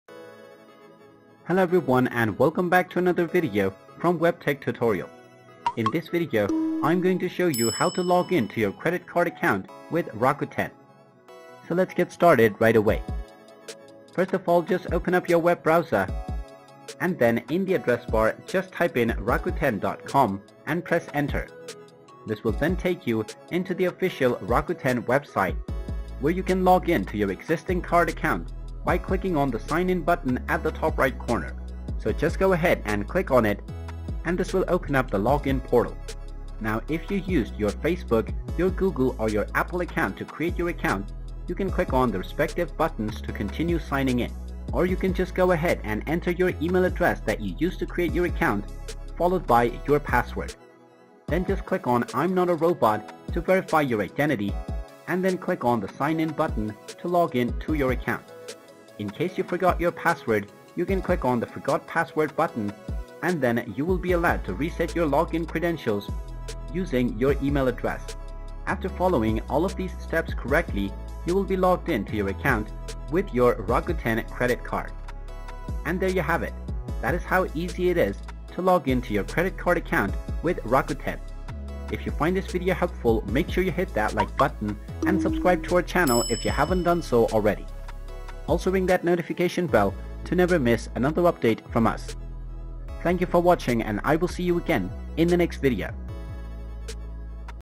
How to Login Rakuten Credit Card Account Online 2023? Sign In Rakuten Credit Card Account
Access your Rakuten Credit Card account effortlessly with our detailed login tutorial. Manage your finances, check rewards, and stay in control. Watch now to streamline your credit card management!